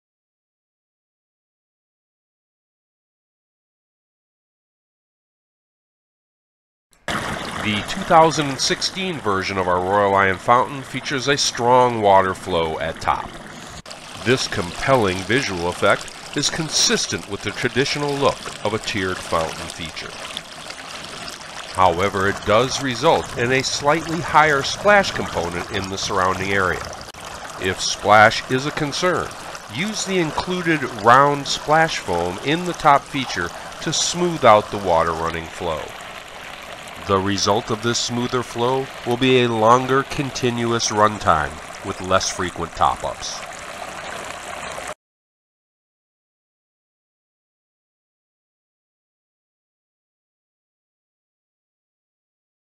Royal Lion Fountain | AD93681| Tech-Tip: Water Flow
Water flow tip for the Royal Lion Fountain AD93681.  An optional foam insert is included with some versions of this fountain.  Its use is detailed in this informative video on how to curb splash and increase fountain run time.